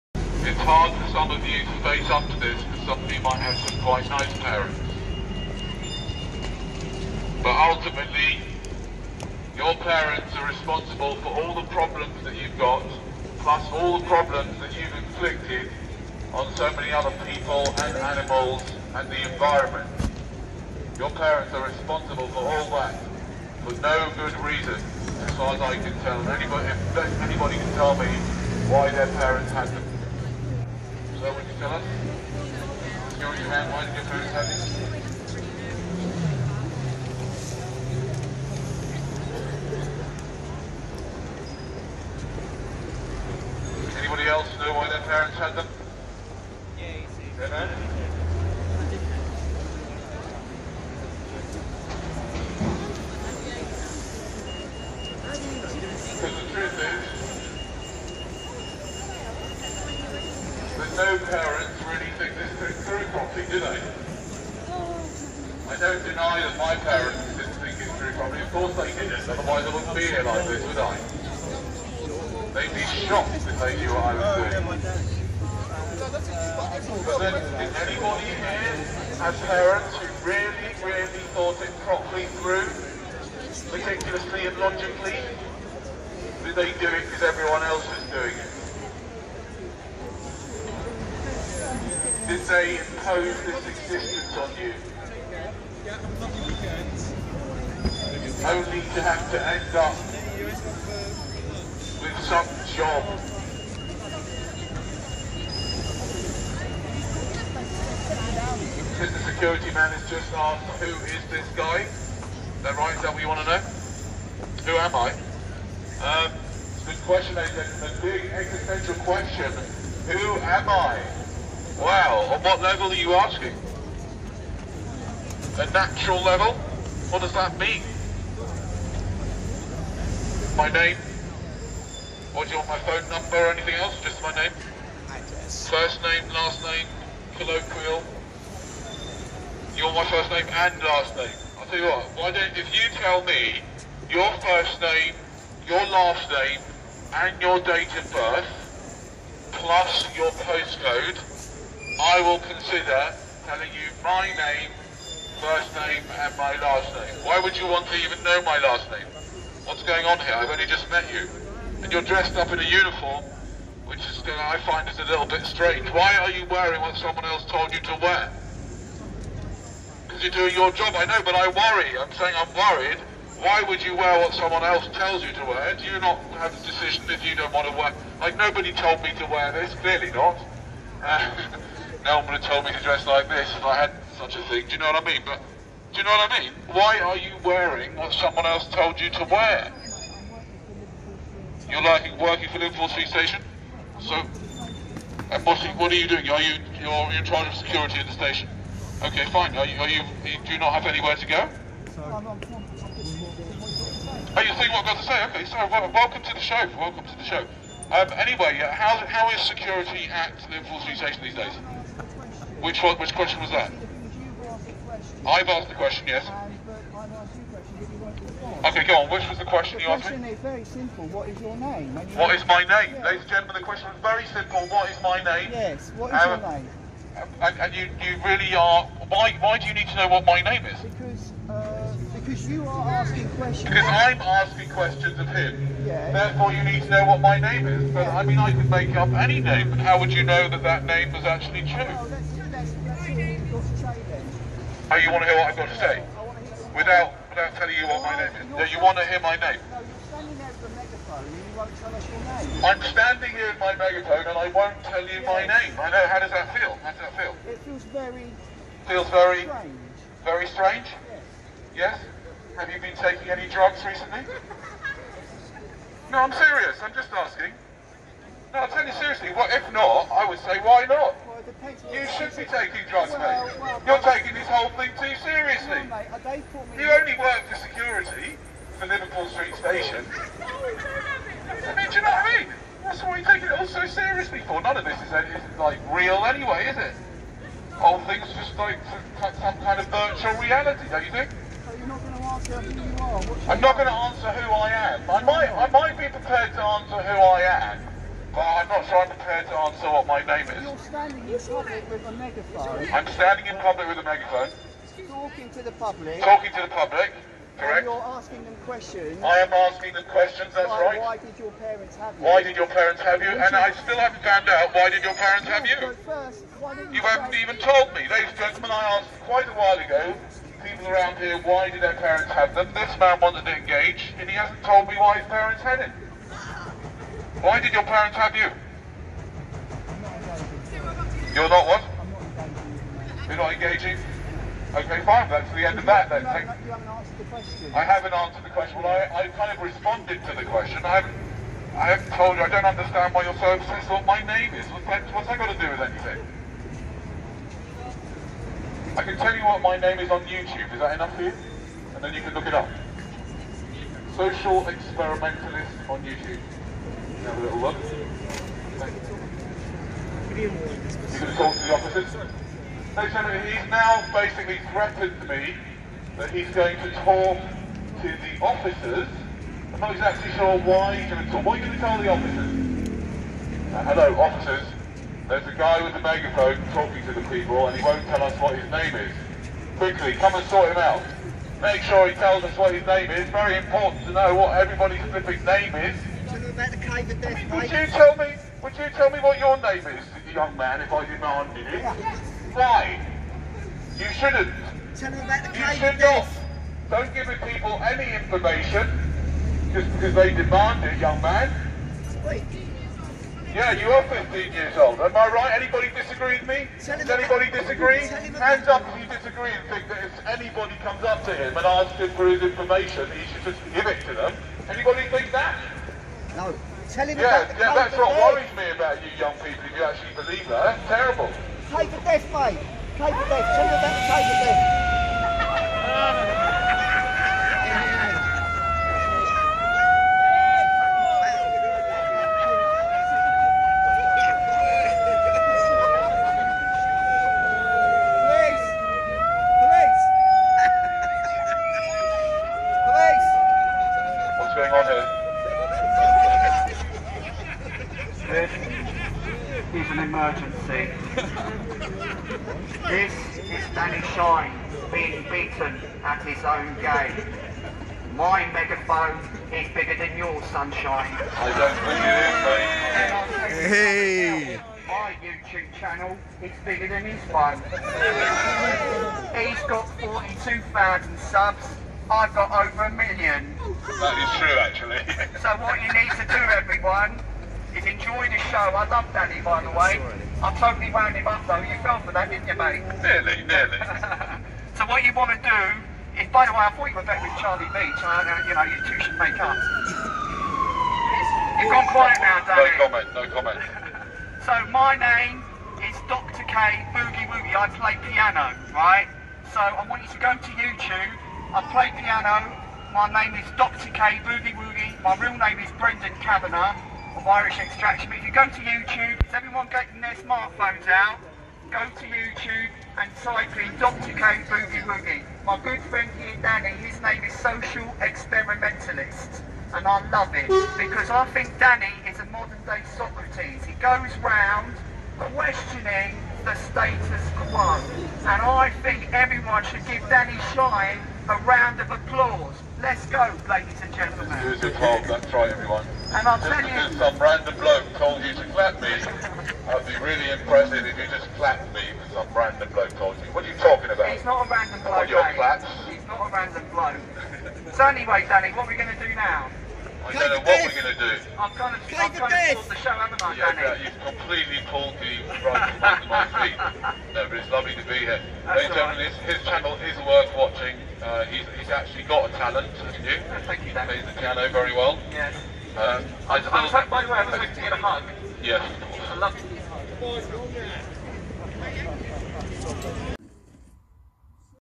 Security guard takes ACTION
Security guard taking it all really seriously or is he?

Joined by Brendan Kavanagh aka Dr K